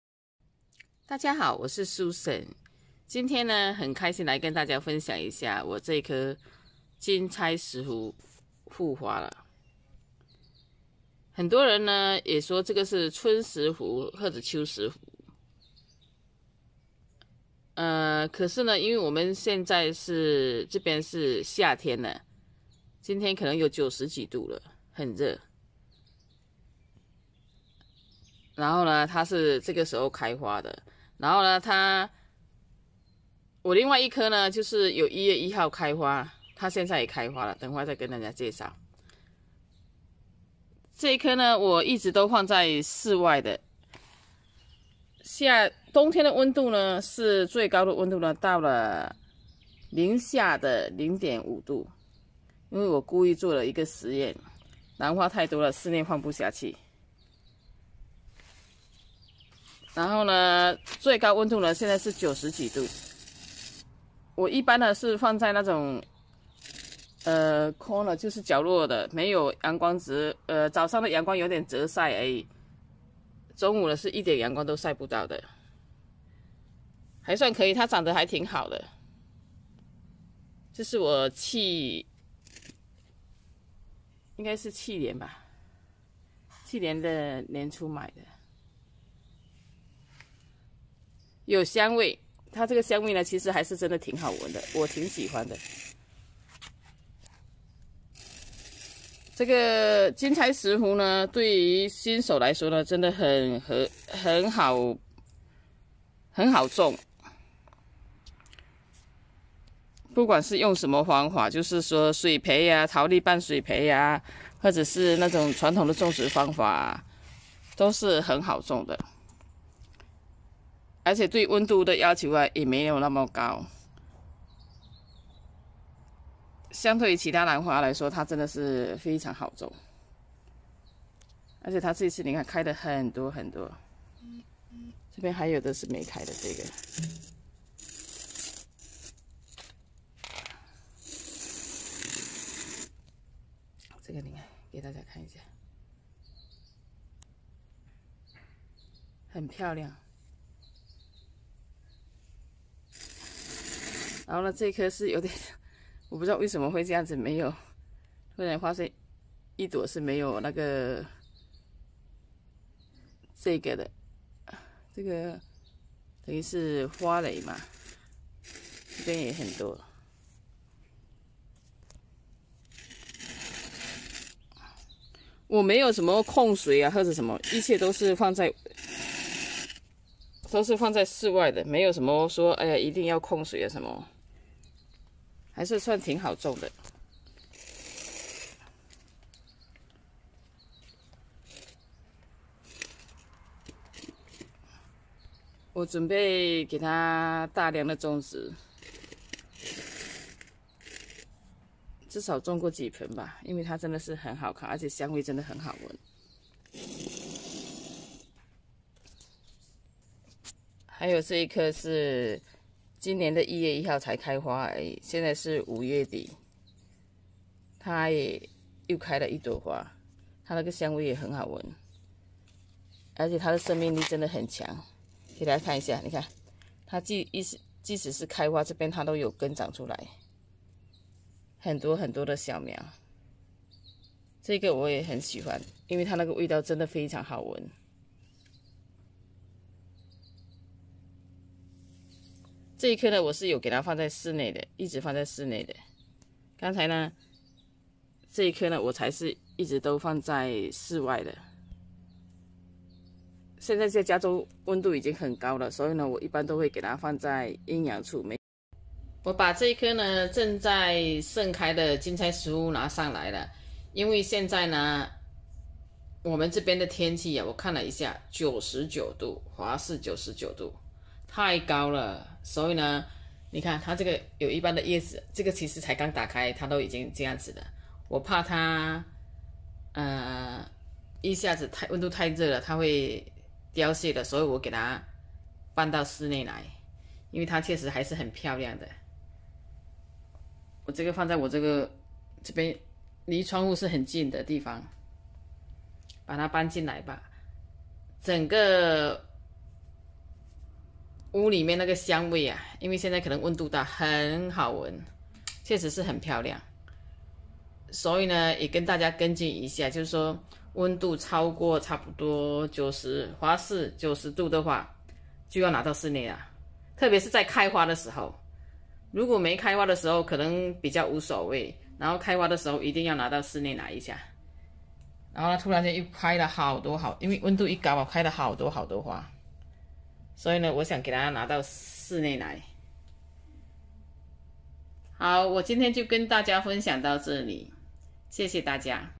兰花156.金钗石斛兰复花及种植环境 05/26/2020
金钗石斛一年开花两次，最低温度零下0.5度，最高温度32度。在花期最好在室内，温度太高花容易凋谢。